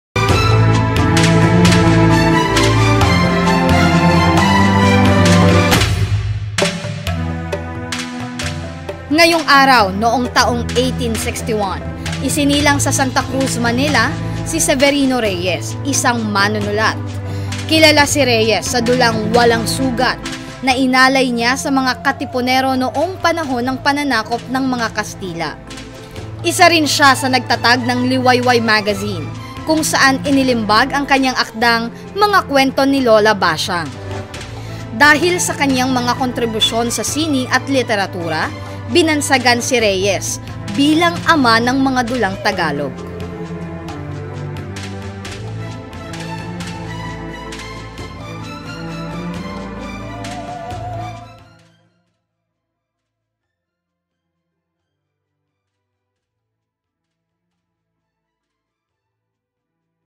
TODAY IN HISTORY - FEBRUARY 11, 1861 | Birth of Severino Reyes
"AMA NG MGA DULANG TAGALOG"